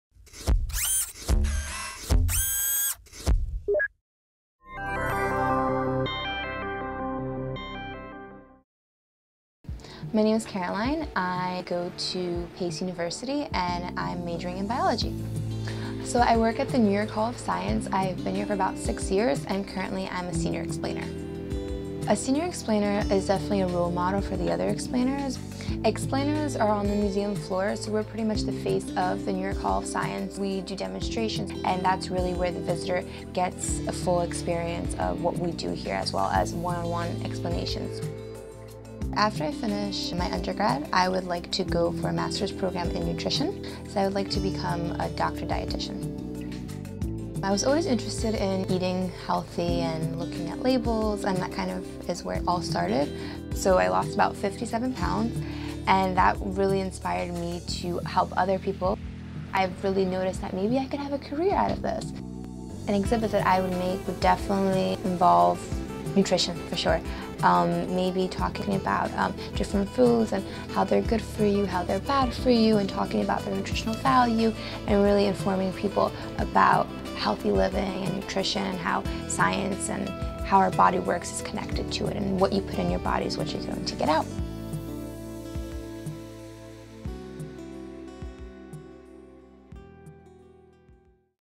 Explainer Spotlight - Caroline Placzek | Explainer TV
This is the story of Caroline Placzek. She is a Senior Explainer and future Dietician. To learn more about Caroline and her weight-loss journey, watch the video.

Credits

Post Production: Valeria Aucapina
Camera 1: Valeria Aucapina
Camera 2: Mike Wilson aka Coma Niddy